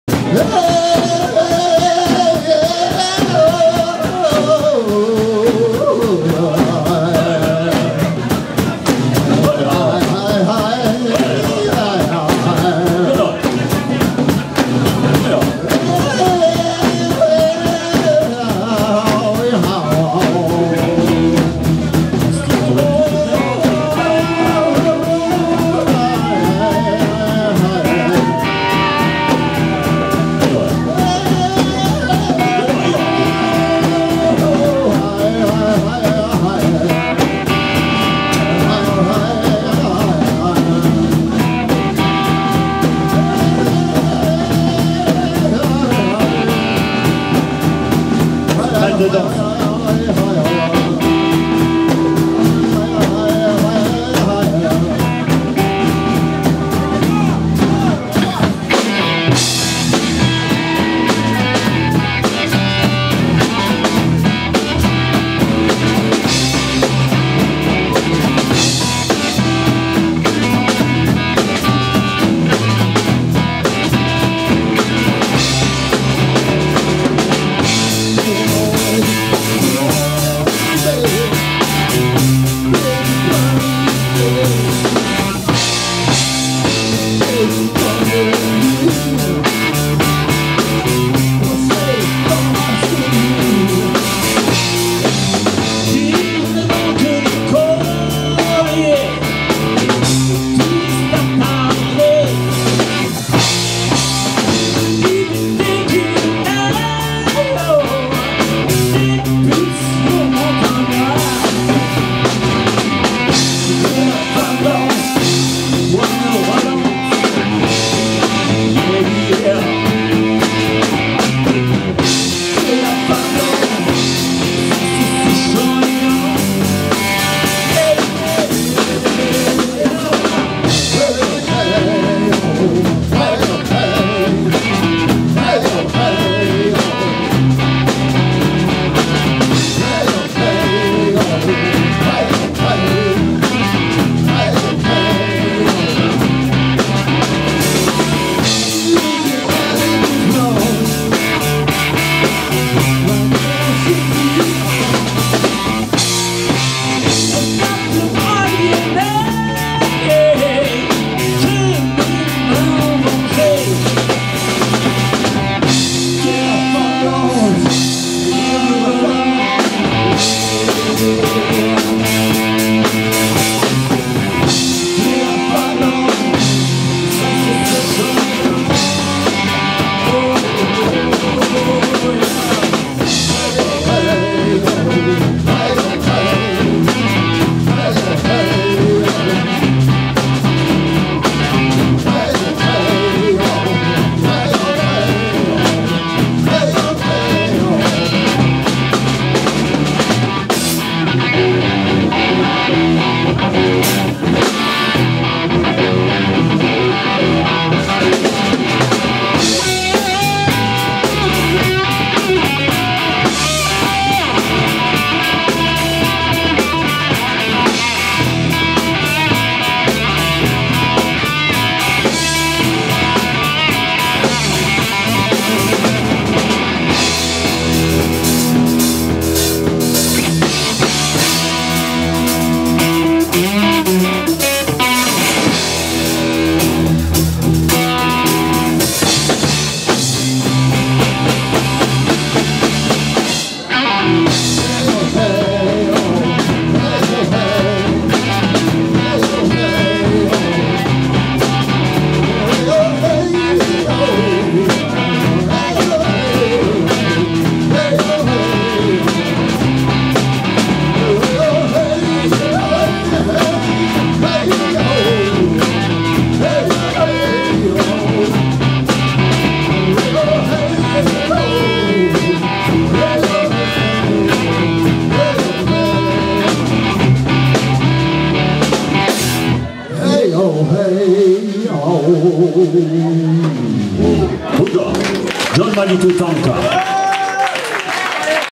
Les Maraudeurs
En concert au V and B le 1er octobre 2016 à Montluçon